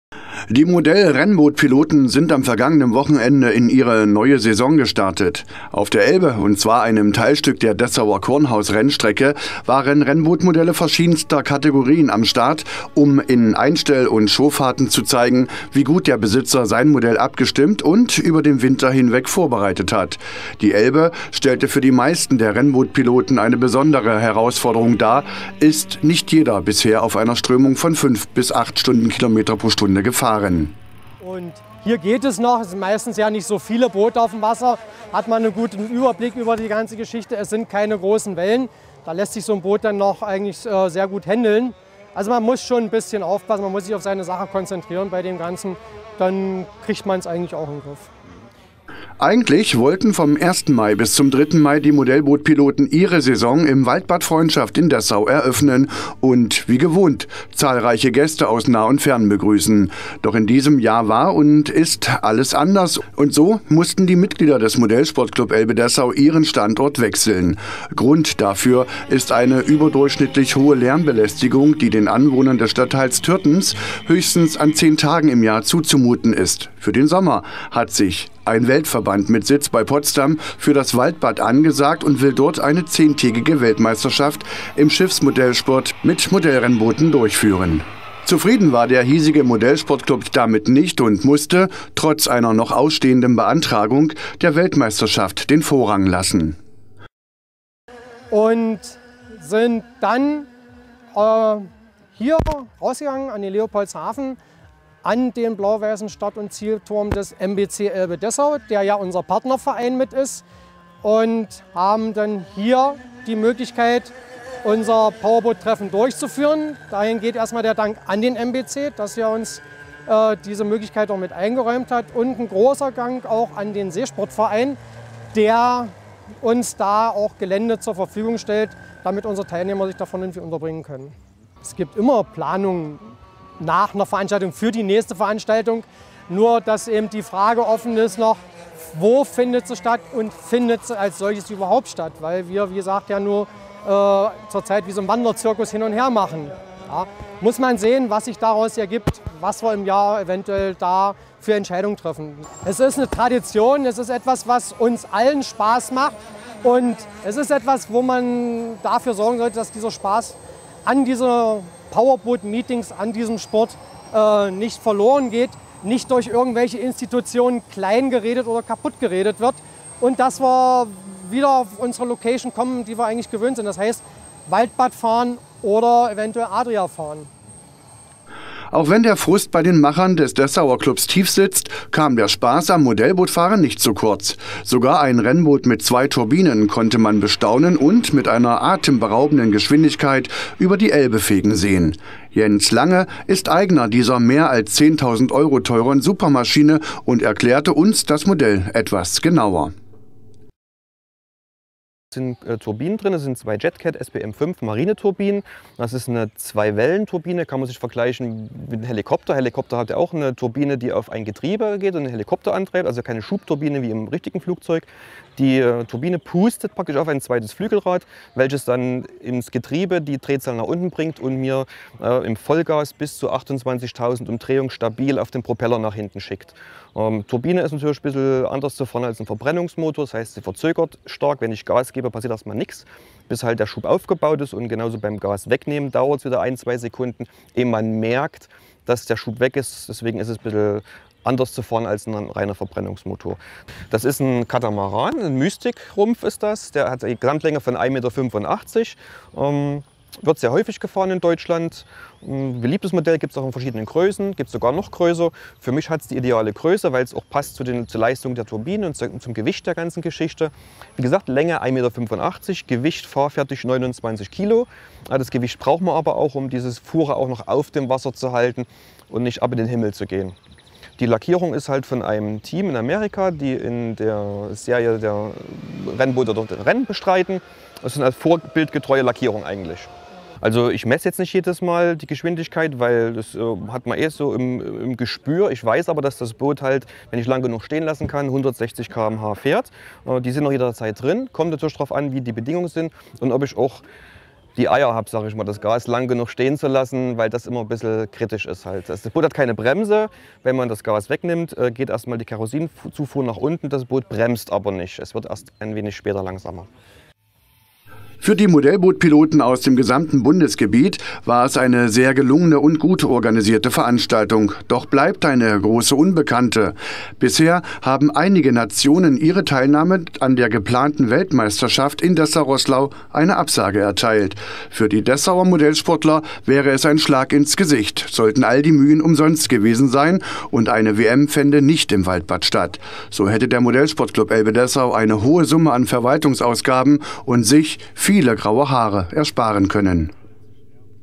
Saisoneröffnung Modellboote - RAN1
Die Modellbootpiloten haben am vergangenem Wochenende ihre Saison eröffnet. Nach langem Hin- und Her, was die Standortwahl anging, haben sich die Mitglieder nur drei Tage vor der Veranstaltung für die Elbe als Austragungsort entschieden. Neben enormen finanziellen Einbußen, die der Verein dadurch zu tragen hat, erfuhren die Modellbootpiloten aber auch Anerkennung von den mehr als 50 Teilnehmern.